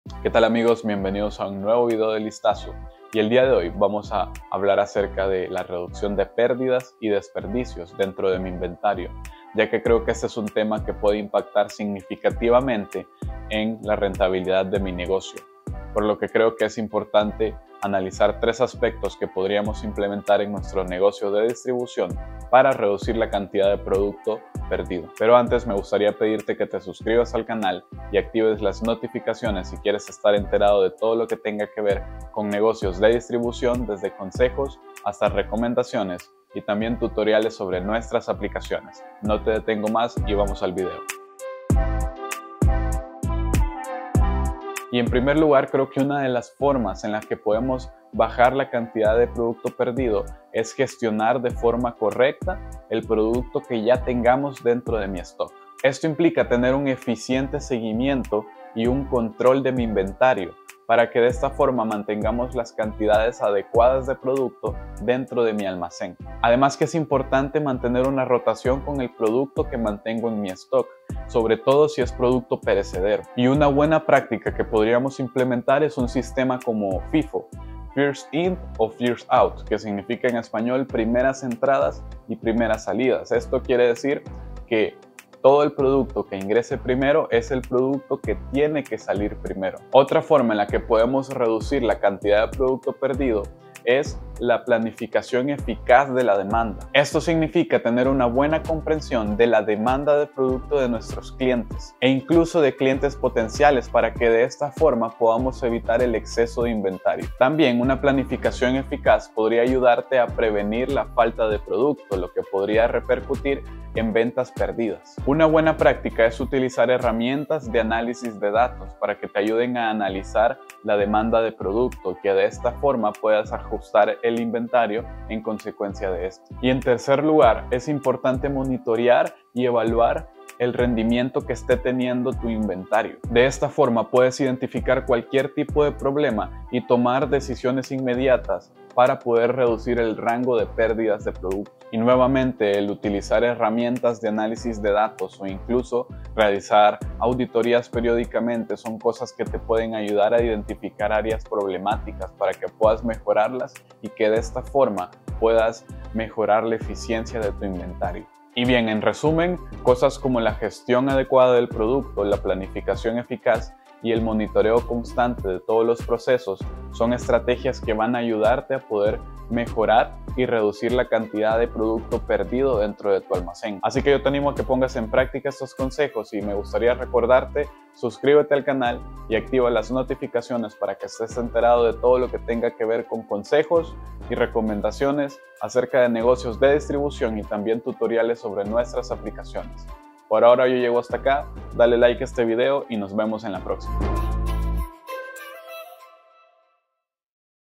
Reducción de pérdidas y desperdicios
En este video, te enseñaremos cómo reducir las pérdidas de inventario a través de una gestión adecuada de los productos en stock. Esto implica tener un sistema eficiente de seguimiento y control de inventarios, implementar la rotación adecuada de inventario y utilizar herramientas de análisis de datos para predecir la demanda y ajustar el inventario en consecuencia.

Además, es importante monitorear y evaluar constantemente el rendimiento del inventario para identificar cualquier problema y tomar medidas inmediatas para reducir las pérdidas. En Listaso, te recomendamos realizar auditorías periódicas de inventario para identificar áreas problemáticas y mejorar la eficiencia de la gestión de inventario.

Con estos consejos, podrás reducir las pérdidas de inventario y optimizar la gestión de tus productos en stock.

0:00 Introducción
0:50 Gestión adecuada de los productos en stock
1:34 Planificación eficaz de la demanda
2:08 Monitoreo y evaluación constante
2:38 Conclusión